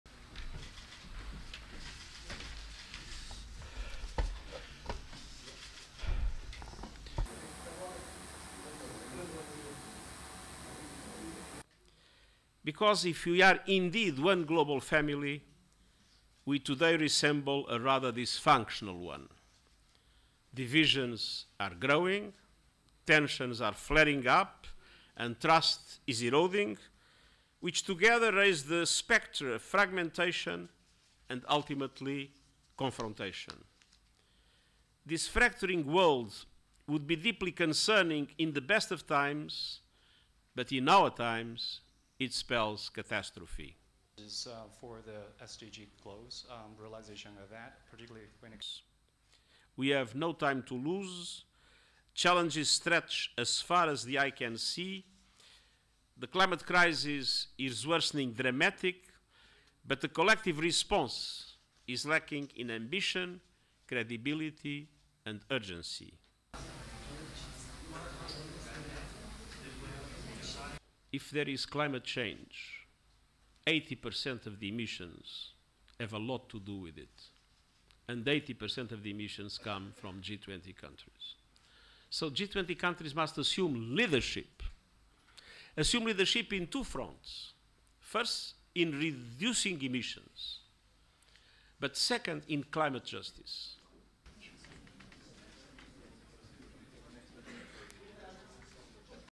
Guterres urges G20 powers to use weekend summit to send strong message on climate change
RESTRICTION SUMMARY:

ASSOCIATED PRESS
New Delhi, India -  08 September 2023
1. Wide of United Nations Secretary General Antonio Guterres arriving for the news conference
2. Mid of UN Flag
3. SOUNDBITE (English) Antonio Guterres, United Nations Secretary General:
"Because if we are indeed one global family – we today resemble a rather dysfunctional one. Divisions are growing, tensions are flaring up, and trust is eroding – which together raise the spectre of fragmentation, and ultimately, confrontation. This fracturing would be deeply concerning in the best of times – but in our times, it spells catastrophe."
4. Mid of news conference
5. SOUNDBITE (English) Antonio Guterres, United Nations Secretary General:
"We have no time to lose. Challenges stretch as far as the eye can see. The climate crisis is worsening dramatically – but the collective response is lacking in ambition, credibility, and urgency."
6. Mid of a cameraman
7. SOUNDBITE (English) Antonio Guterres, United Nations Secretary General:
"If there is climate change, eighty percent of the emissions have a lot to do with it. And eighty percent of the emissions come from G20 countries. So G20 countries must assume leadership, assume leadership in two fronts, first in reducing emissions but second in climate justice."
8. Wide of Guterres leaving

STORYLINE:
U.N. Secretary-General António Guterres on Friday urged the Group of 20 top economic powers, which are responsible for more than 80% of the emissions that cause global warming, to use their weekend summit to send a strong message on climate change.

"If there is climate change, eighty percent of the emissions have a lot to do with it. And eighty percent of the emissions come from G20 countries," Guterres said at a news conference at the U.N. office in New Delhi.

"So G20 countries must assume leadership, assume leadership in two fronts, first in reducing emissions but second in climate justice," he added.

Earlier this year, the U.N. weather agency had said that there’s a two-out-of-three chance that the world will temporarily hit a key warming limit within the next five years.

July 2023 was Earth’s hottest month on record by a wide margin.

Climate ministers of the G20 nations ended their last meeting for the year in July without resolving major disagreements on climate policies.

“The climate crisis is worsening dramatically — but the collective response is lacking in ambition, credibility, and urgency,” said Guterres.

Guterres called on big emitters to make additional efforts to cut emissions and rich countries to meet the climate finance commitments made already.

Meanwhile, India's priorities for the G20 summit include efforts to develop alternative fuels like hydrogen, resource efficiency and reforming development banks like the World Bank and International Monetary Fund to help make funds more accessible for lower- and middle-income countries as they seek solutions to combat climate change.

“If we are indeed one global family,” the U.N. chief said, referring to India’s theme for the meetings, ”we today resemble a rather dysfunctional one.”

As leaders gathered for the weekend summit, Russia's war on Ukraine threatened to dominate the talks, with its effect on food and energy security along with other global implications.